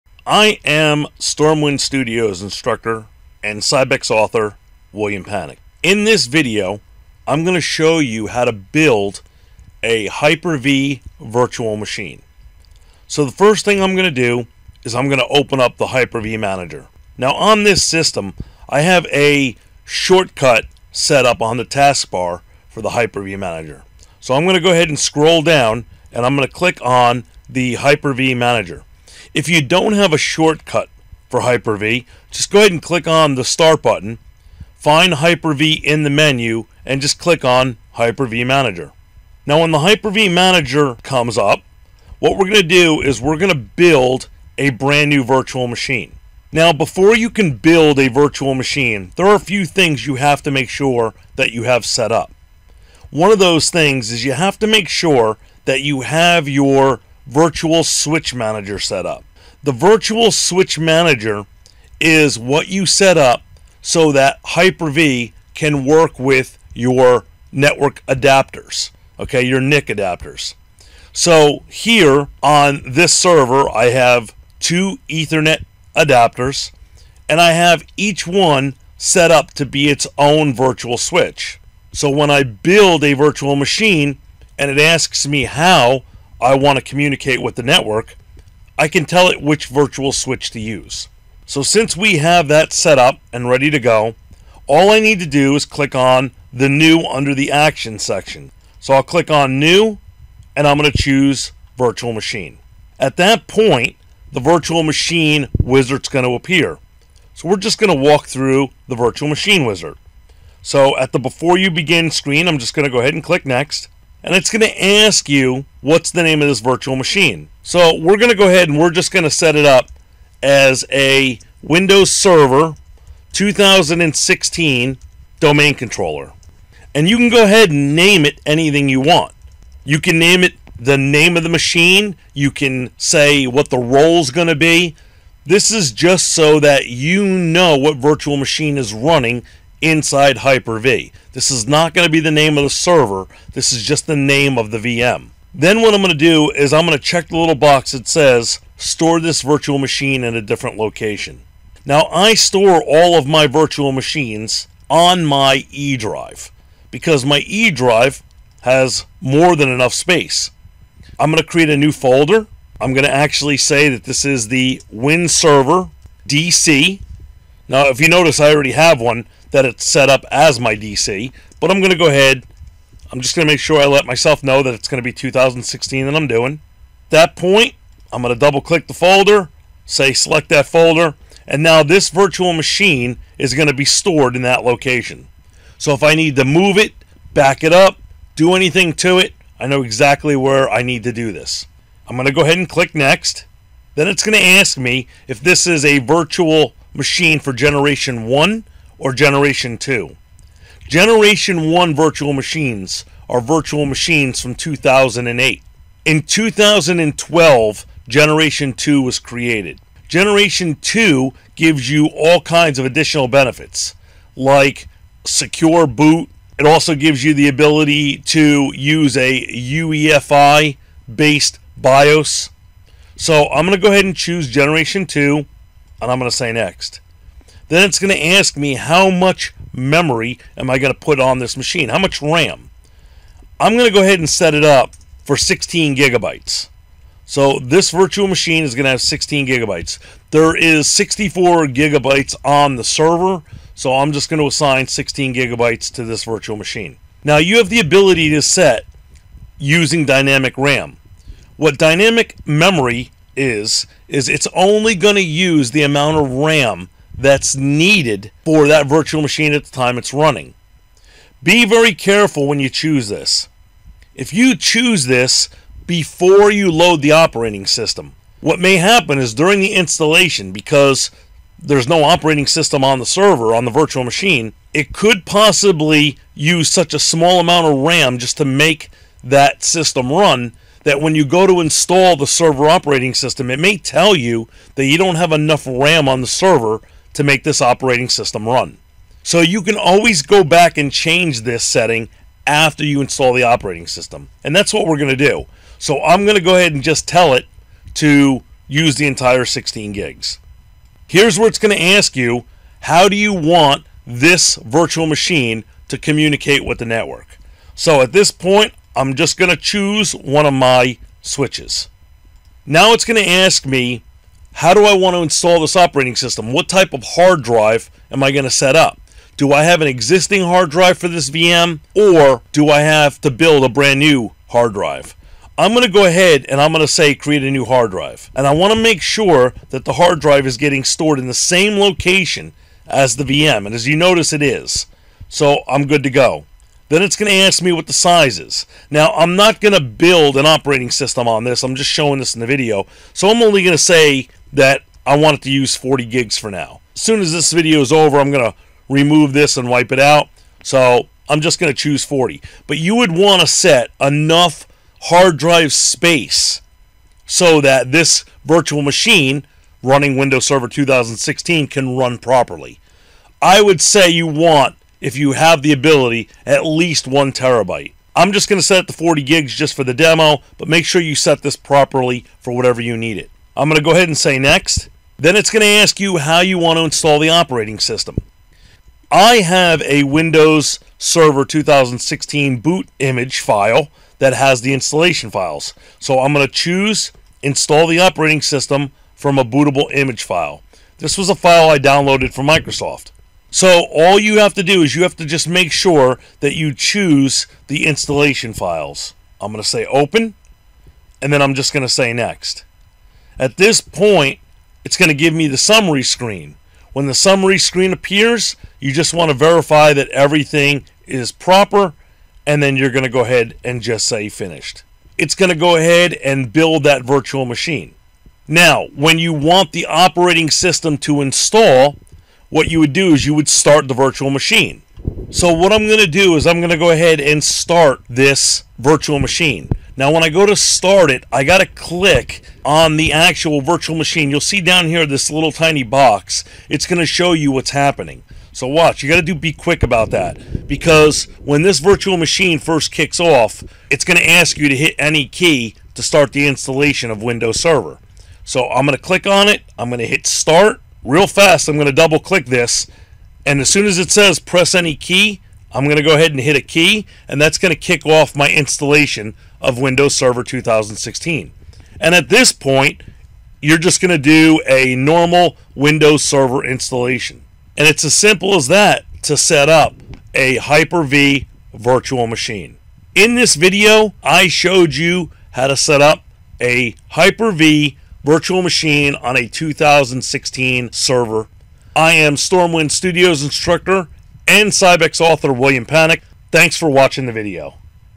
Creating a new VM on Win Server 2016 Hyper-V
In this video, StormWind Studios instructor and Sybex Author Will Panek shows you how to create a new virtual machine in Windows Server 2016 Hyper-V. Will shows you how to setup the VM and how to load a Windows Server 2016 operating system into the VM.